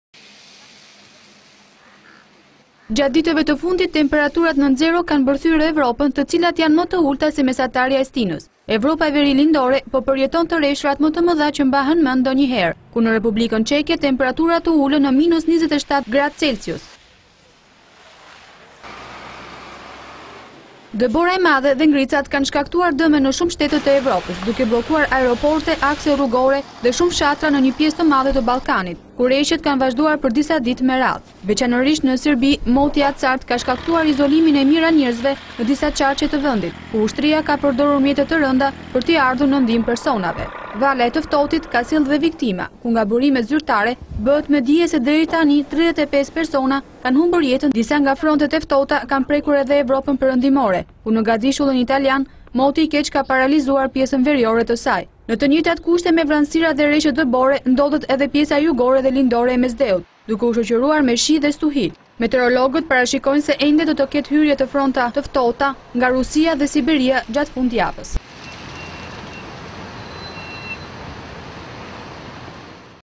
Evropë, reshjet e dëborës bllokojnë aeroportet dhe akset rrugore
Gjatë ditëve të fundit temperaturat nën zero kanë mbërthyer Evropën, të cilat janë më të ulta se mesatarja e stinës. Evropa do të vazhdojë të jetë në ndikimin e fronteve të ftohta nga Rusia dhe Siberia. Stuhitë dhe reshjet e dëborës kanë bllokuar aeroportet dhe akse rrugore. Ka njoftime për viktima në disa vende.
Në Evropën verilindore janë shënuar nivelet më të larta të reshjeve të borës të regjistruara ndonjëherë. Në Repubblikën Ceke temperaturat u ulën në minus 27 gradë Celsius.